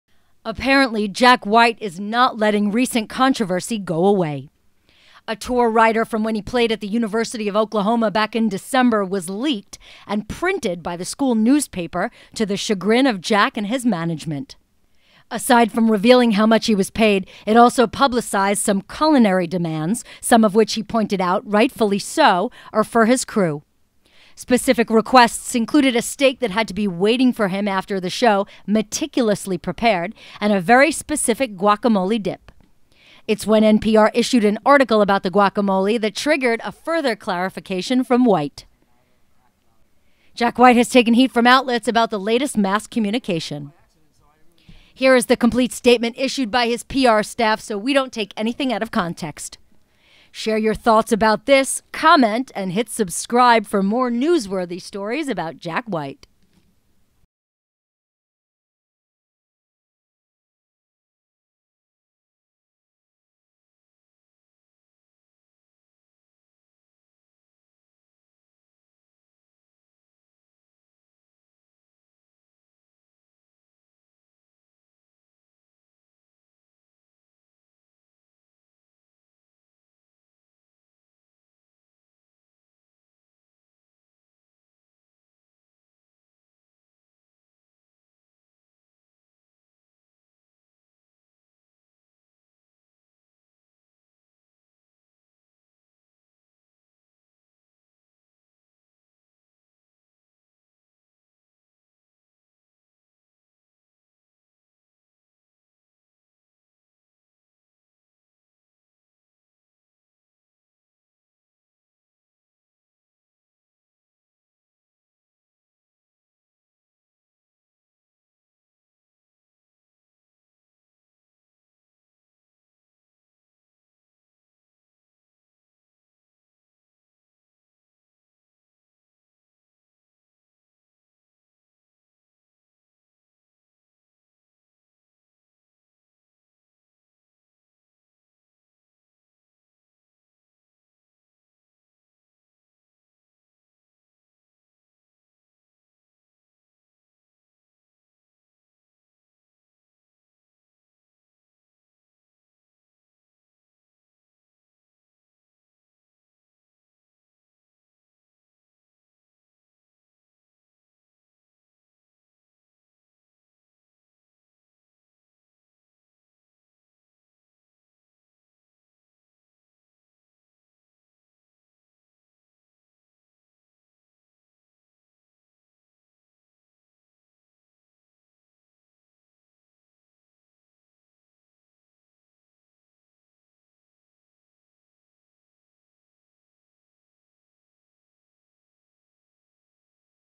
JACK WHITE FULL LETTER TO PRESS, FANS, IN REFERENCE TO TOUR RIDER LEAK
Instead of letting the reports of his tour rider eventually fade away, Jack White has taken the matter into his own hands addressing a letter to the press and public about several controversial requests in his leaked tour rider in Oklahoma including a meticulous recipe for guacamole, and other request - see what he has to say in its entirety.